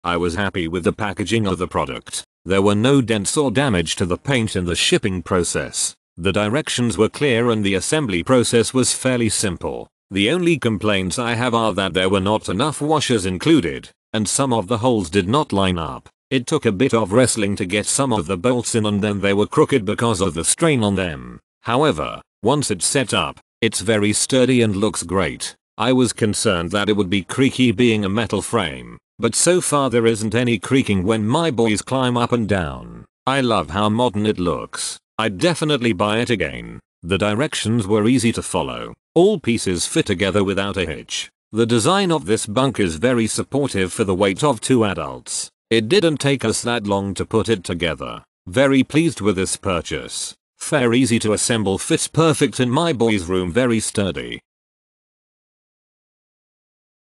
Coaster Home Furnishings Bunk Bed, 82.25"W x 46"D x 65.25"H, Dark Gunmetal review
Coaster Home Furnishings Bunk Bed, 82.25"W x 46"D x 65.25"H, Dark Gunmetal

Main Features:
- Set includes: One (1) bunk bed
- Materials: Steel
- Finish Color: Gunmetal
- Bed size: Twin over twin
- Assembly Required: Yes